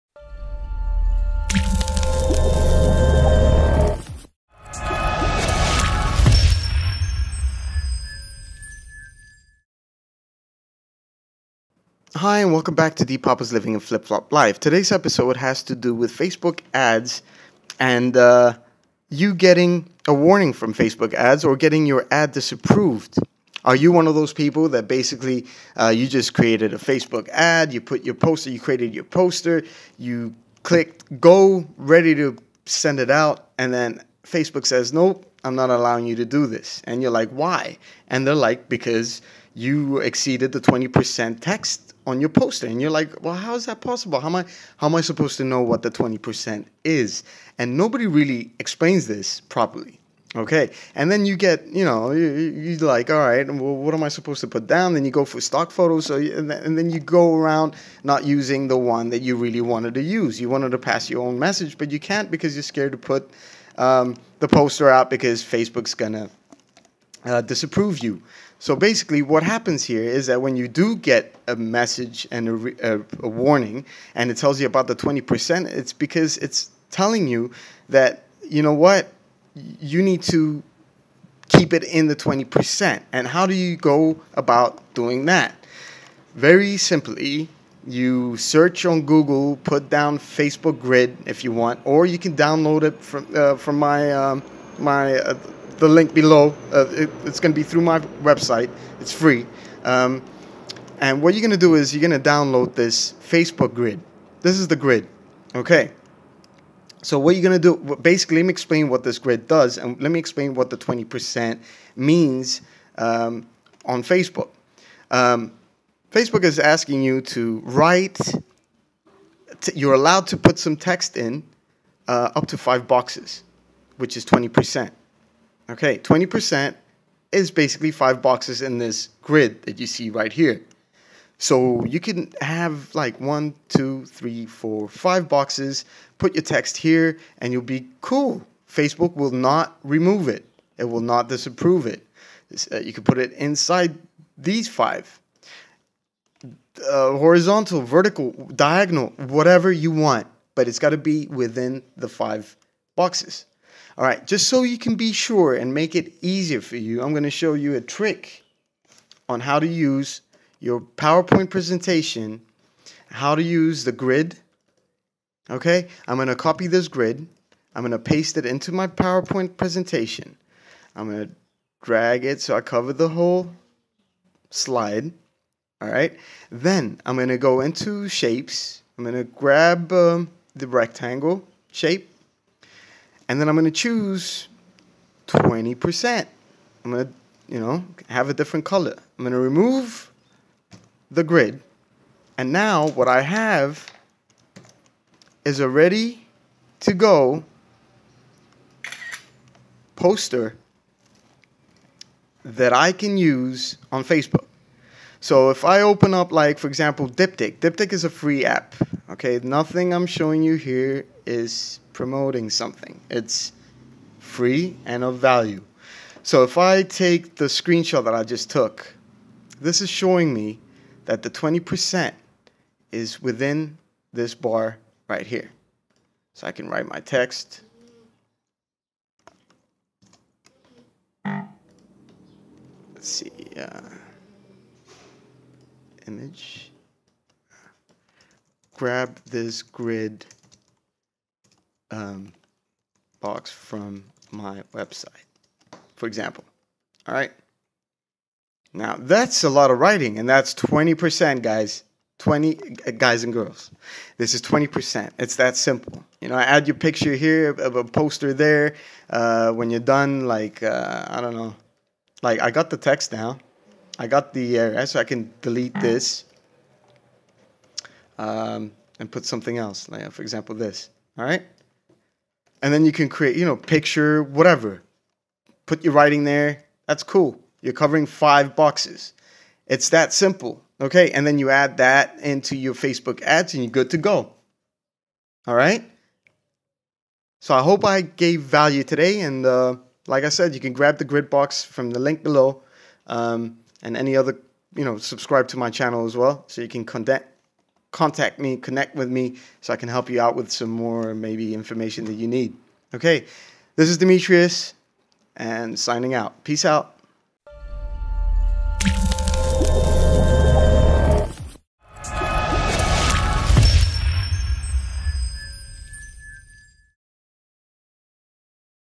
Facebook Disapproved your AD again? Here's your Solution to the 20% Text Limitation
So Facebook Disapproved your Ad Again, due to the text exceeding its 20% limitation on its advertising policy.  Your solution is in this video.